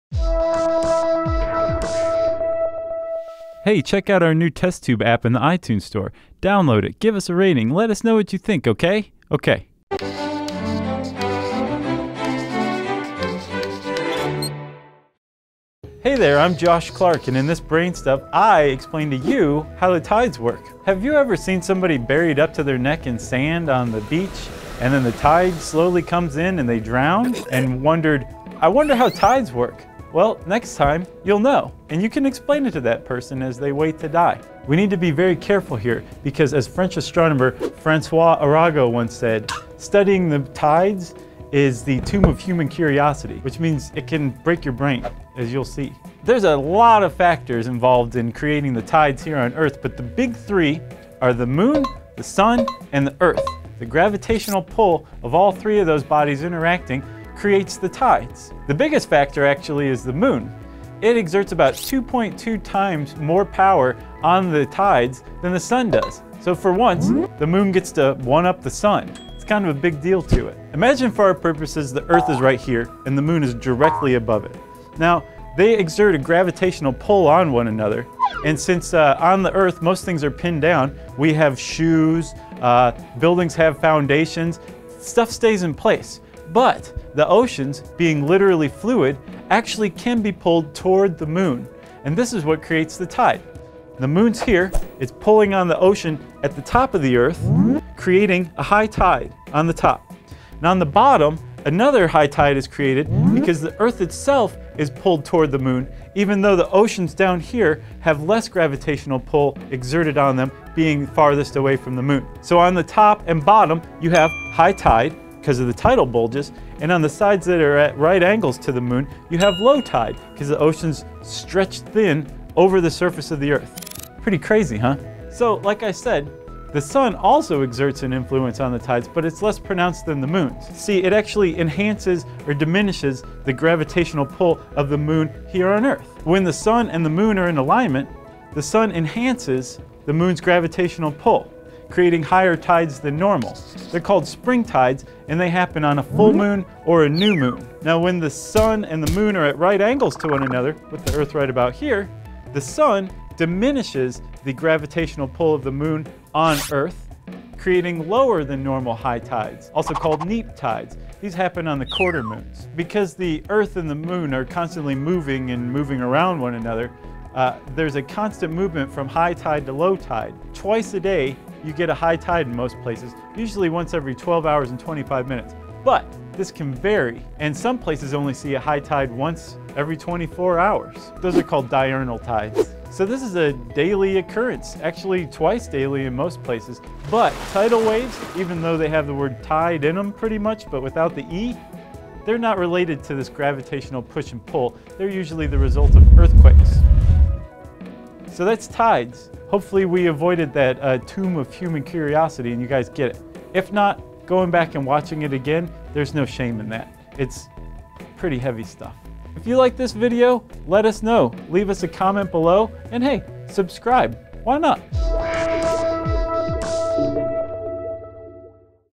How Do Tides Work?
Most people are already familiar with the idea of high and low tides, but where do they come from? What causes this movement and why does it occur with such regularity? Tune in as Josh demystifies the rise and fall of the sea.